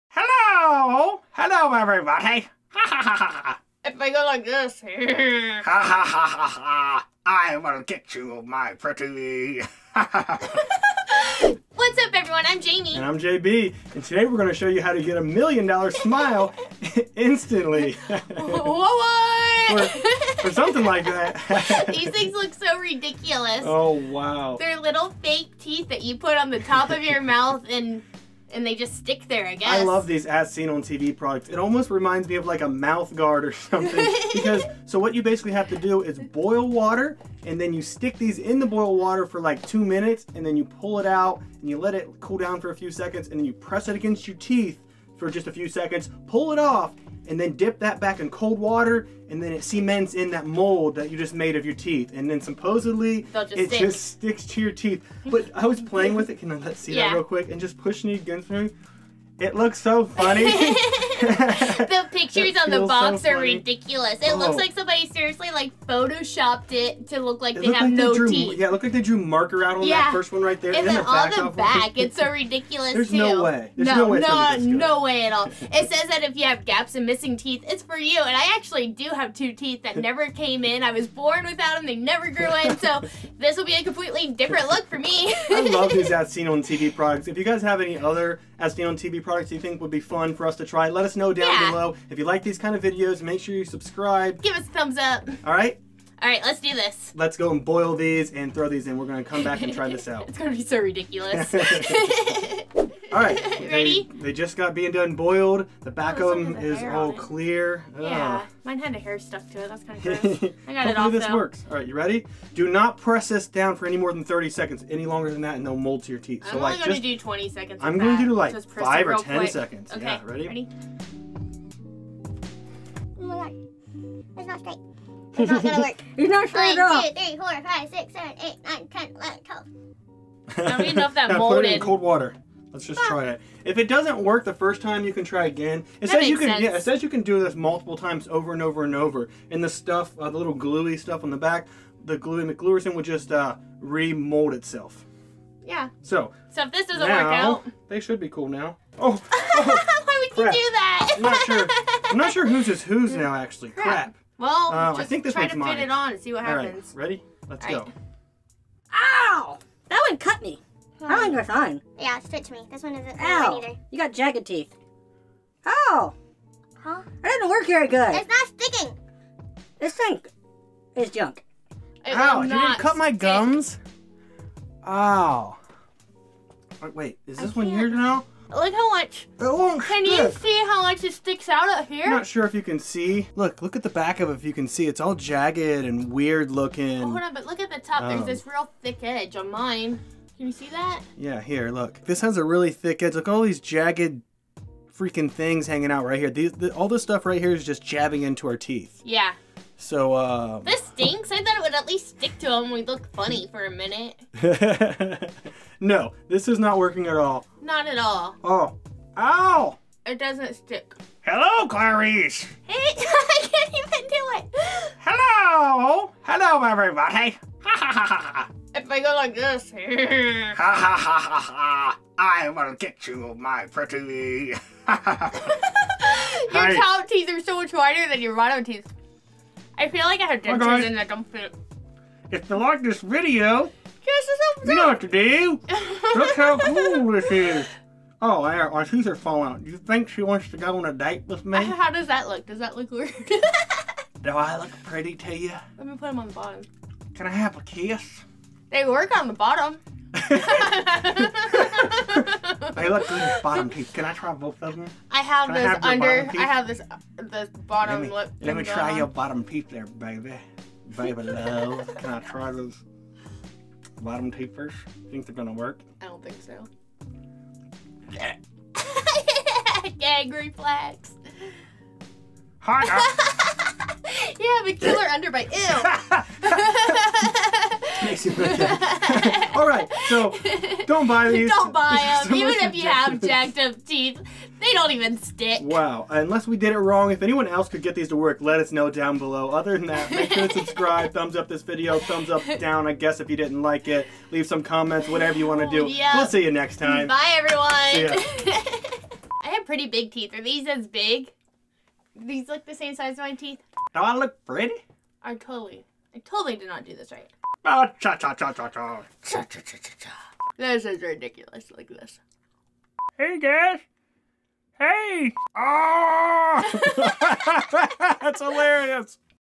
Perfect Smile Veneers | As Seen On Tv Product Review | What!? What!?
Today we have another As Seen On Tv Product called Perfect Smile Veneers. Do these things actually work or is it just a waste of money? Let's find out!

Follow Jaymie on SnapChat - @jaymiesrandom

Camera Gear:
Main Camera
Second Camera
Audio Recorder